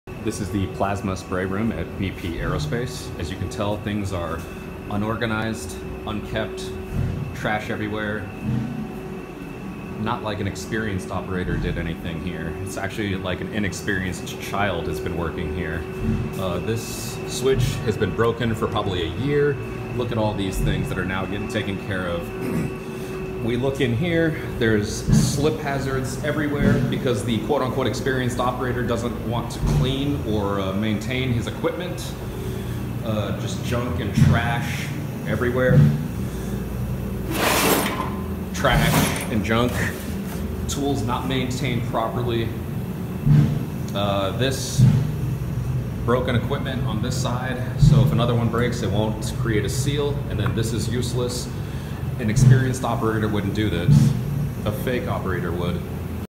Reality of Some Aerospace Companies - When the person in charge doesn’t give a sh*t
The reality is that many aerospace companies are trash with only the veil of professionalism.

I don’t recommend working at this company.